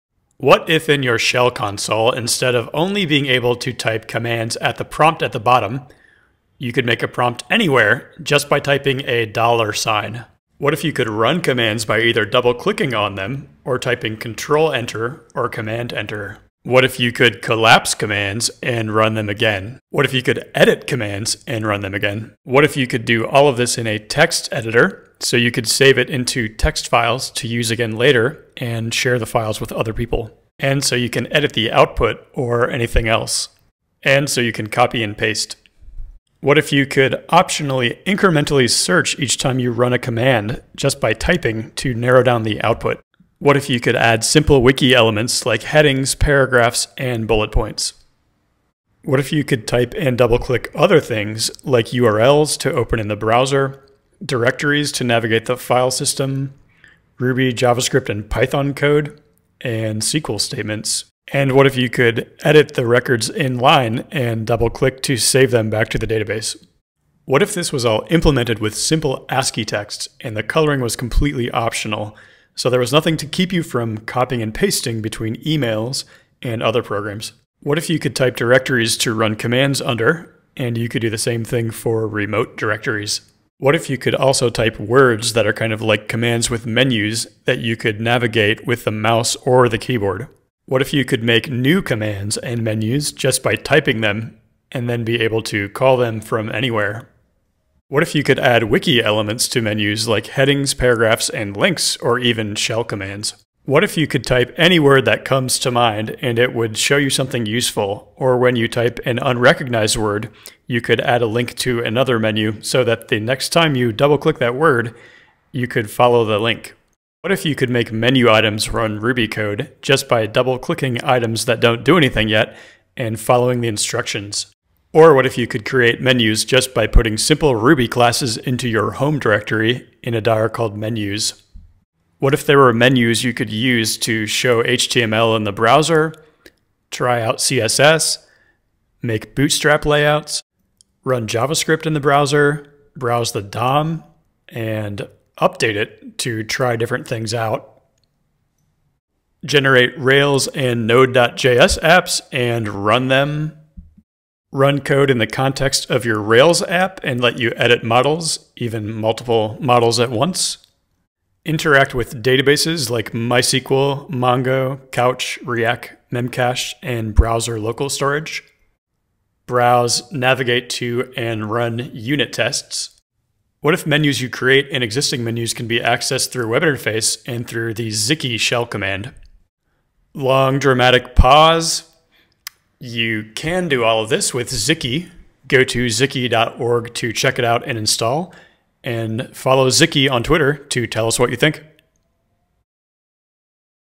Xiki: Can your shell console do this?
A very quick overview of Xiki.  Like a shell console but better.